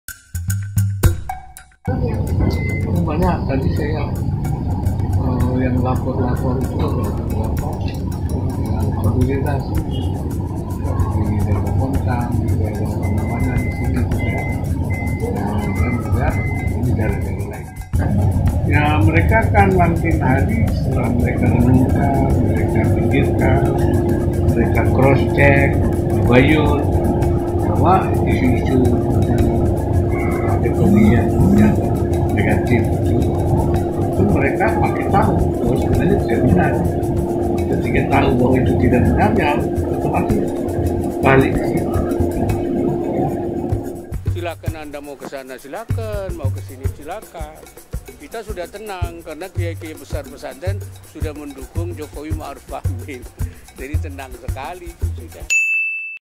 Dukungan Ulama Banten Kepada Jokowi Ma’ruf Amin
Kian hari kian banyak yang mendukung Jokowi-Ma’ruf Amin. Kali ini dukungan datang dari sejumlah ulama Banten. Setelah merenung dan berpikir, mereka memutuskan untuk mendukung pasangan 01. Mereka juga mengetahui bahwa isu negatif yang menyerang Jokowi-Ma’ruf Amin tidaklah benar.